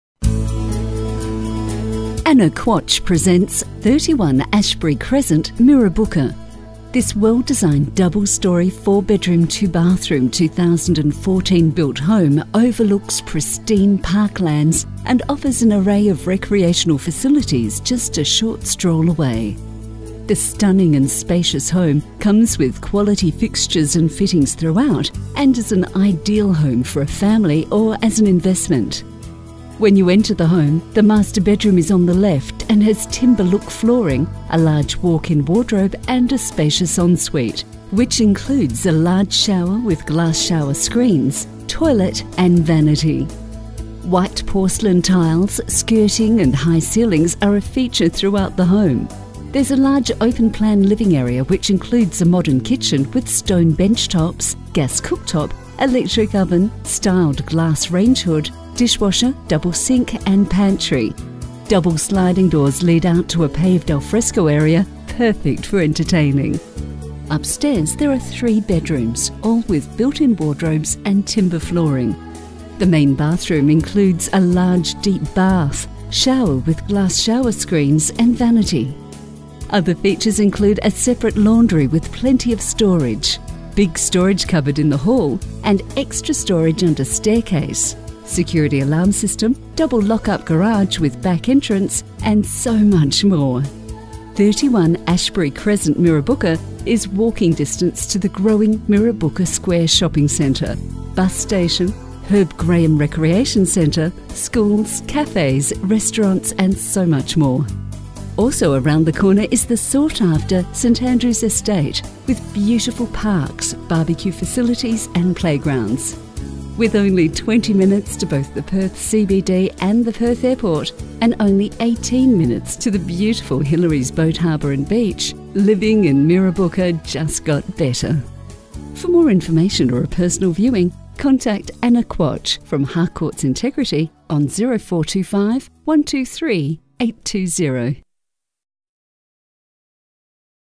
Living in Mirrabooka just got better!
Please turn on your speakers to hear the presentation.

Anna Quach presents 31 Ashbury Crescent Mirrabooka WA
This well-designed double storey 4 bedroom, 2 bathroom 2014 built home, overlooks pristine parklands and offers an array of recreational facilities just a short stroll away.

This stunning and spacious home, comes with quality fixtures and fittings throughout and is an ideal home for a family or as an investment.  31 Ashbury Crescent, Mirrabooka is walking distance to the growing Mirrabooka Square Shopping Centre, Bus Station, Herb Graham Recreational Centre, schools, cafes, restaurants and so much more.

Also around the corner is the sought after St Andrews Estate with beautiful parks, BBQ facilities and playgrounds.   With only 20 minutes to both the Perth CBD and the Perth Airport and only 18 minutes to the beautiful Hillary's Boat Harbour and beach, living in Mirrabooka just got better.

For more information or a personal viewing, contact Anna Quach from Harcourts Integrity on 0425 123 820.